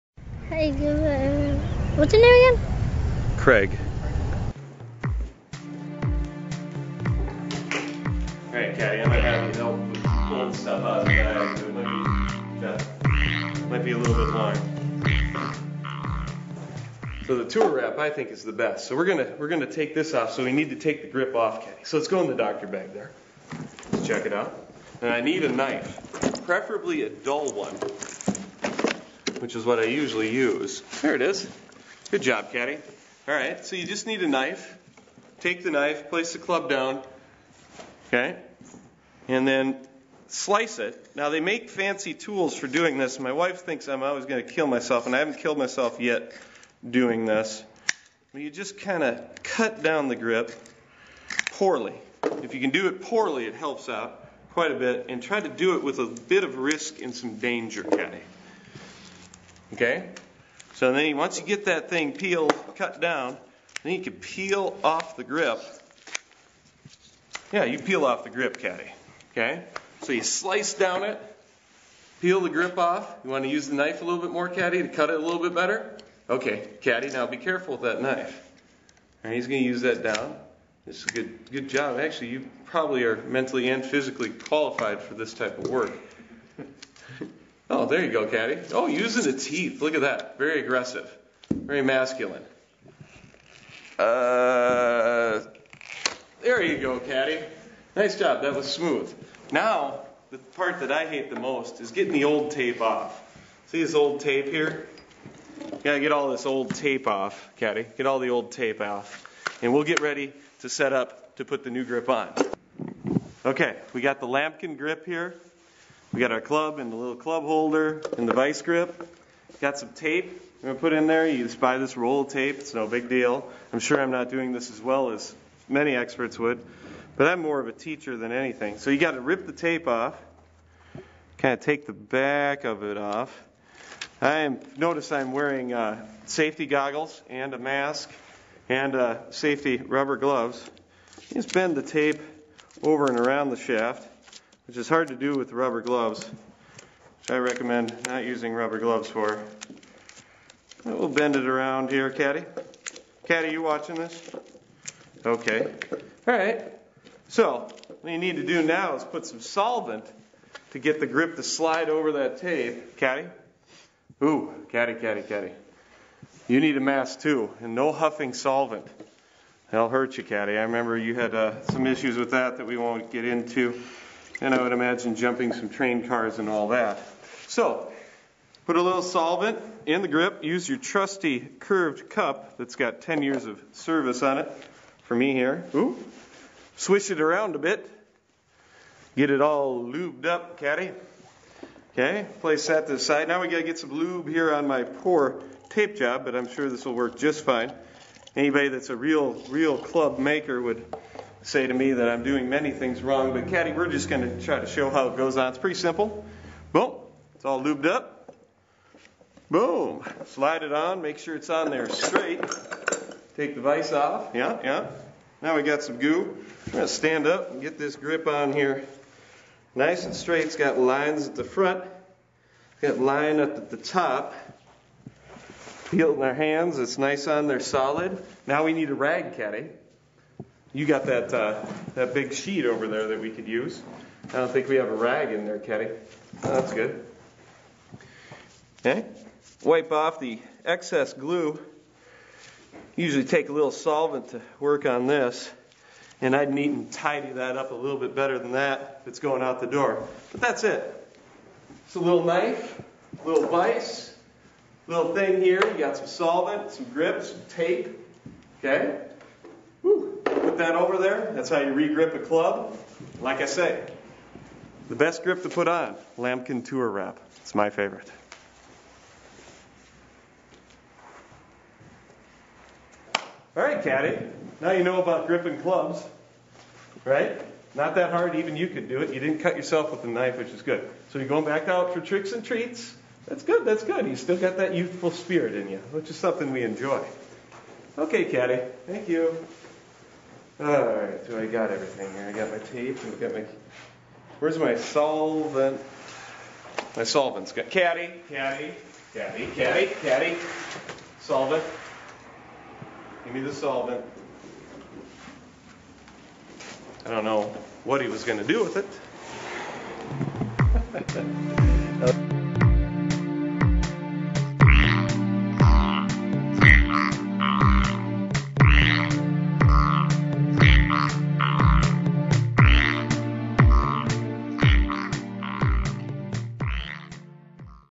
Get a Grip Pt. 2
Like changing your oil or replacing your own bike tube, YOU TOO can change your own golf club grips. Just go easy on the solvent, folks.